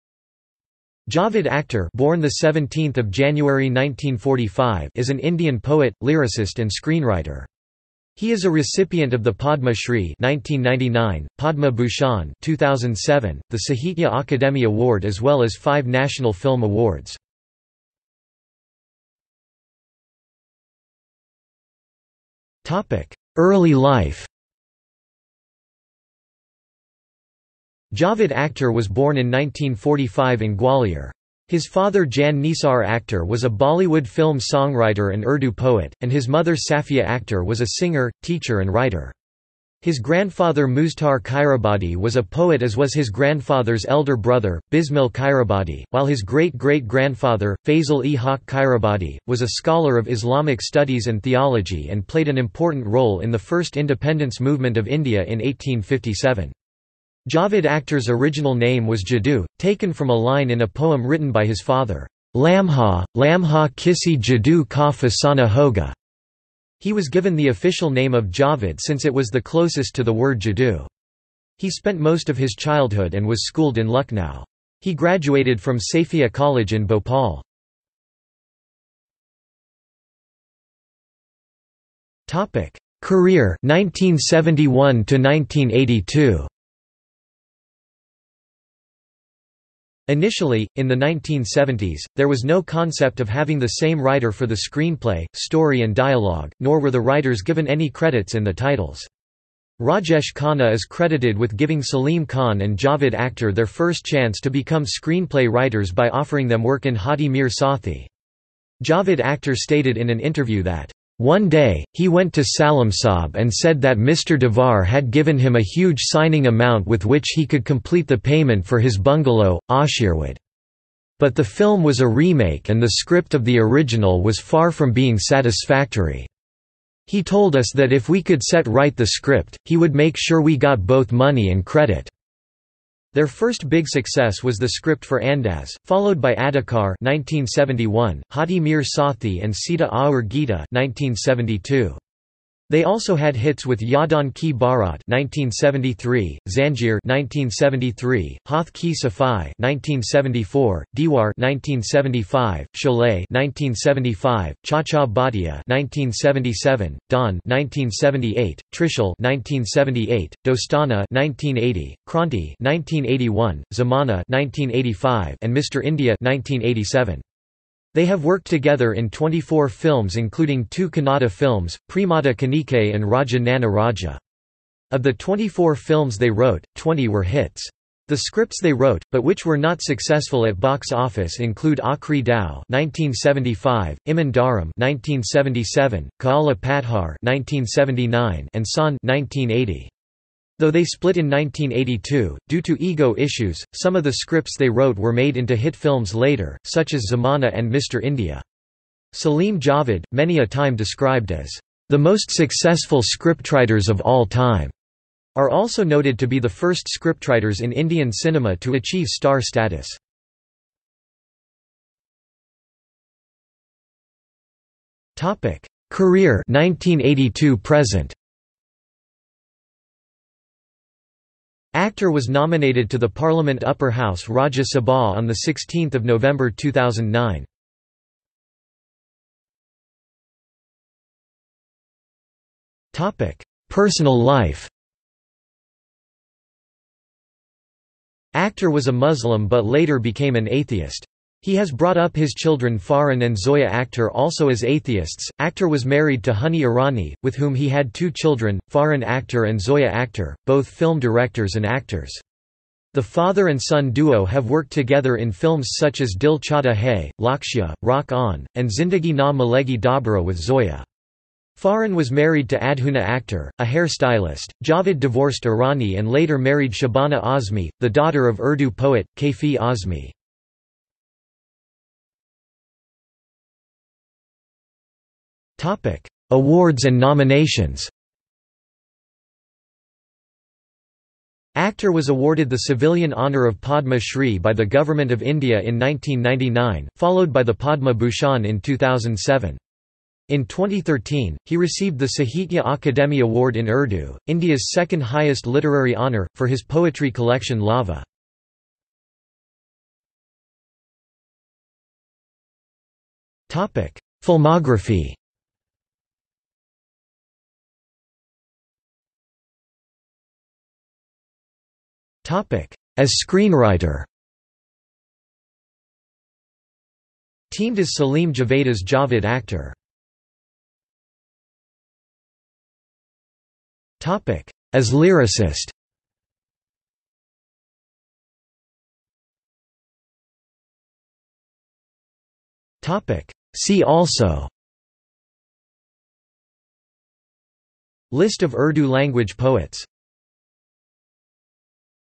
Javed Akhtar | Wikipedia audio article
This is an audio version of the Wikipedia Article:
Javed Akhtar

00:00:19 1 Early life
00:01:18 1.1 Career (1971–1982)
00:03:31 1.2 Career (1982–present)
00:03:49 2 Personal life
00:04:32 3 Awards and nominations
00:04:59 4 Filmography
00:05:08 4.1 As screenwriter
00:05:20 4.2 As lyricist
00:05:29 5 See also

- Socrates

SUMMARY
Javed Akhtar (born 17 January 1945) is an Indian poet, lyricist and screenwriter. He is a recipient of the Padma Shri (1999), Padma Bhushan (2007), the Sahitya Akademi Award as well as five National Film Awards.